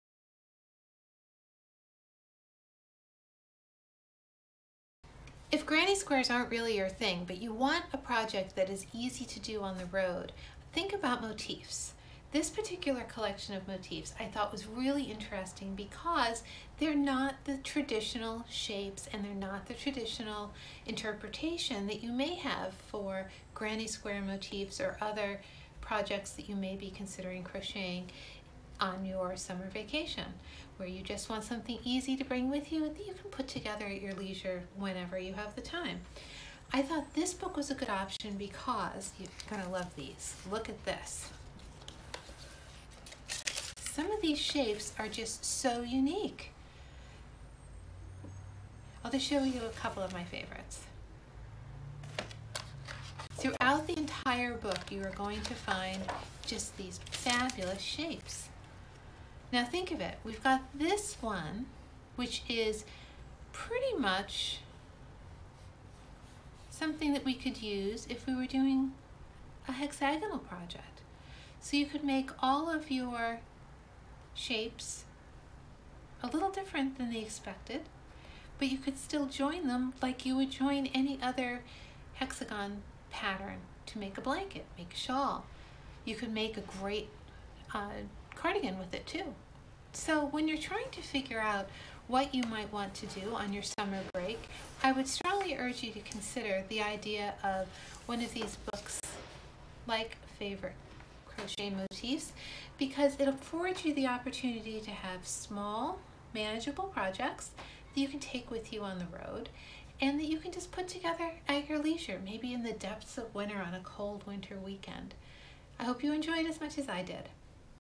Book Review - Add a little Color - Part 2 - Colorful Crochet Every Day - Motifs
Need a few ideas for a non-traditional portable project? Here's a collection of small motifs easy to work up from your stash bag at the beach or poolside sipping mojitos with the girls.

Look at my favorites, let me know what you think.

Where to find the book:

Cozy Coastal Knits